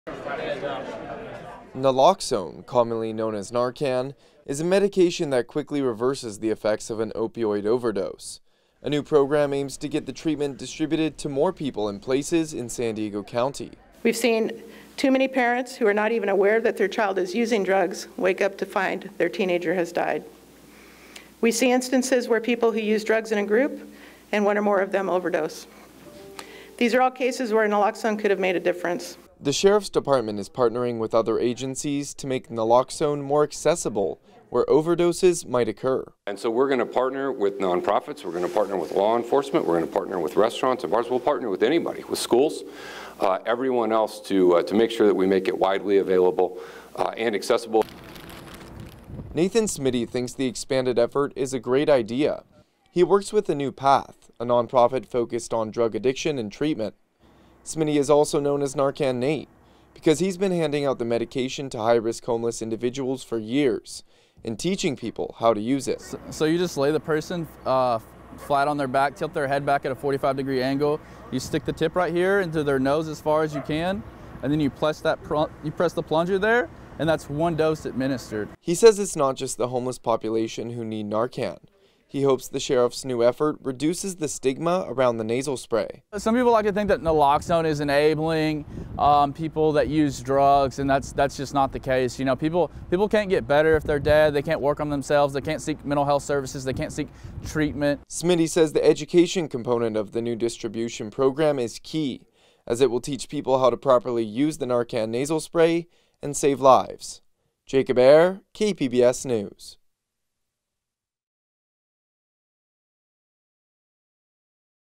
San Diego County Sheriff expands Naloxone distribution to combat opioid overdoses
The San Diego County Sheriff's Department is launching a new effort to get a life-saving medication out to places where opioid overdoses are likely to happen.  KPBS reporter Jacob Aere has the story.